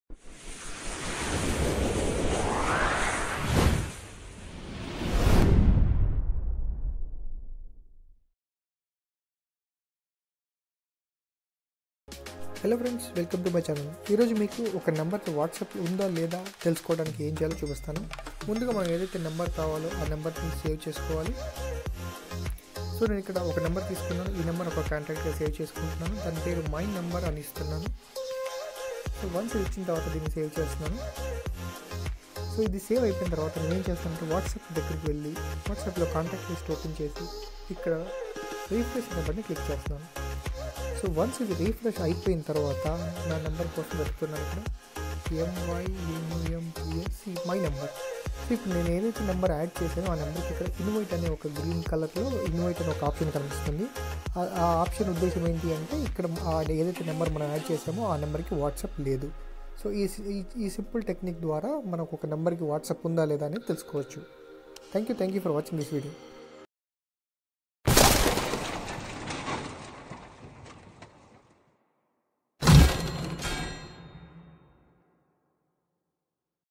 How to find a Number have Whatsapp account or not in easy technique
ఒక నెంబర్ వాట్సప్‌లో ఉందో లేదో ఇలా తెలుసుకోండి..